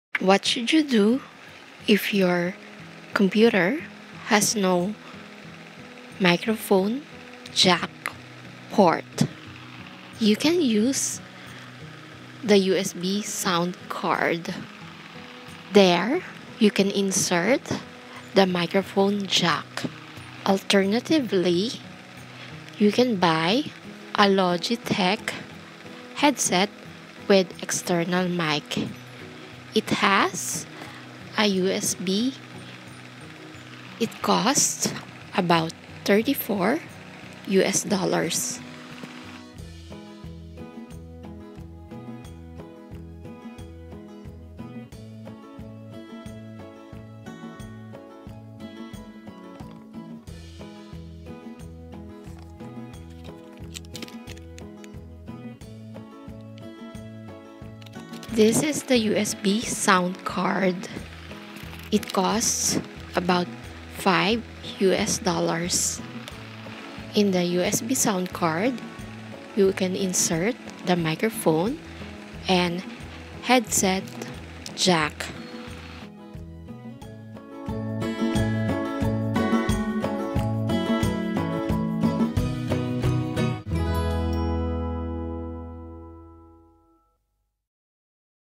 What Should You Do If Your Computer Has No Microphone Jack Port?
Logitech Headset with External Microphone and USB Sound Card